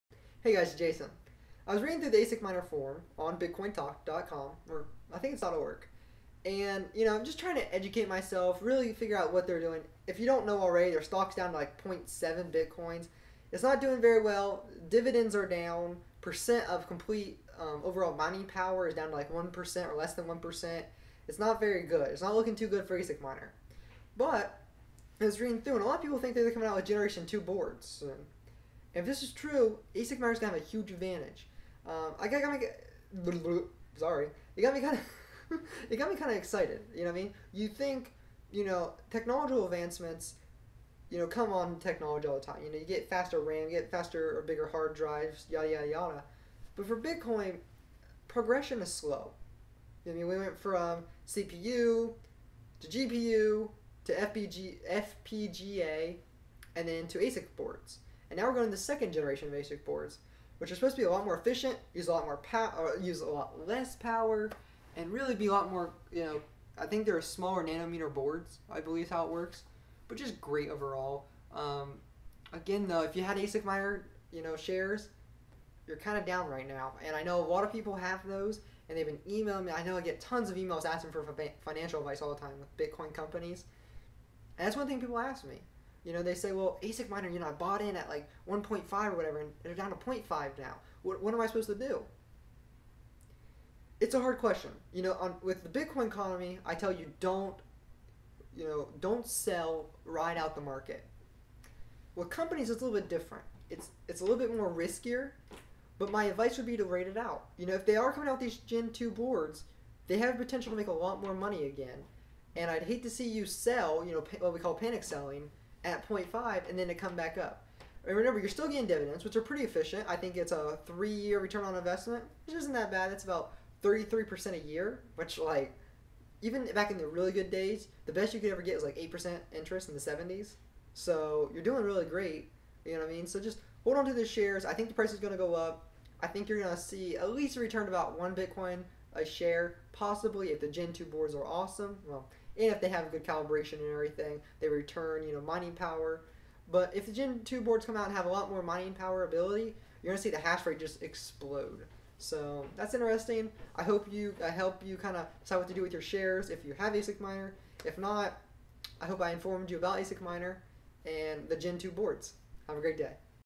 ASICMiner Stock Plummets, High Prices Coming?
In this video, I discuss about how ASICMiner has decreased in value and how with gen 2 boards could possibly push the price back up.